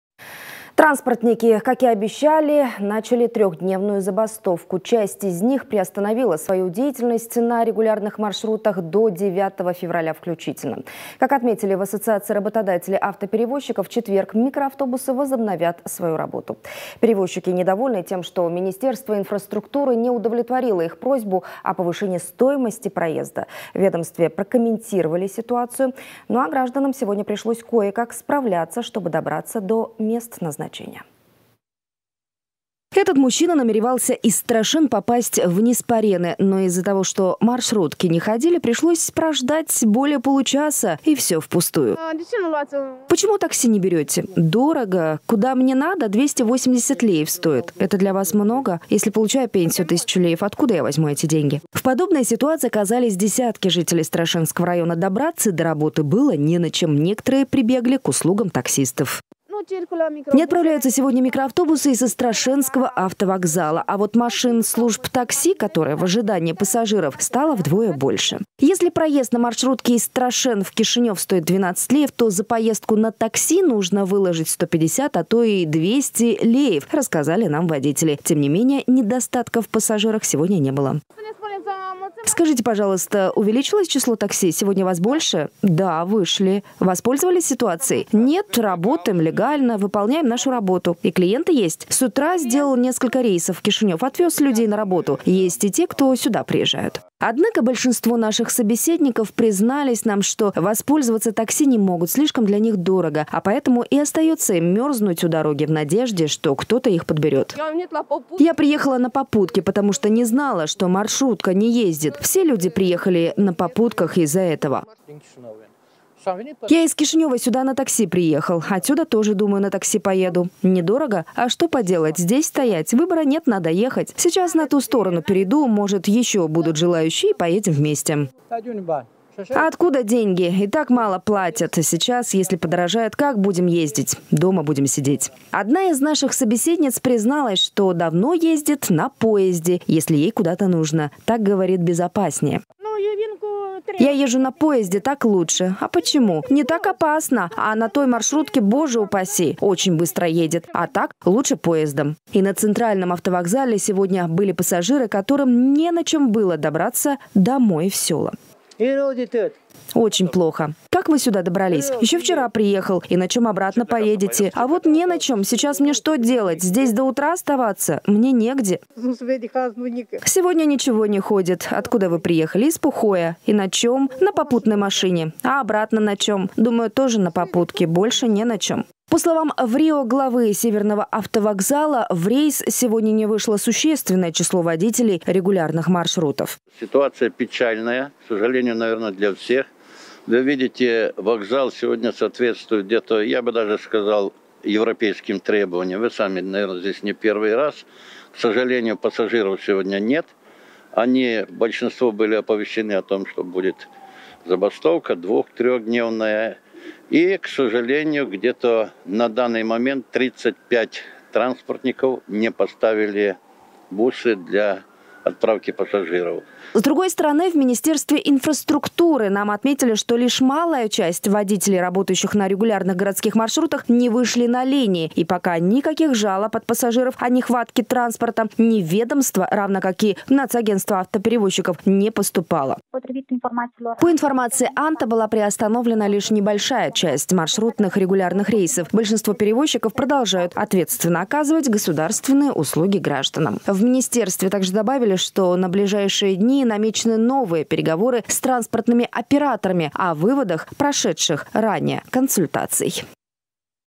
Без междугороднего транспорта. Некоторые транспортники бастуют до 10 февраля
Транспортники, как и обещали, начали трехдневную забастовку. Часть из них приостановила свою деятельность на регулярных маршрутах до 9 февраля. Как отметили в Ассоциации работодателей автоперевозчиков, в четверг микроавтобусы возобновят работу. Перевозчики недовольны тем, что Министерство инфраструктуры не удовлетворило их просьбу о повышении стоимости проезда. В ведомстве прокомментировали ситуацию, ну а гражданам сегодня пришлось кое-как справляться, чтобы добраться до места назначения.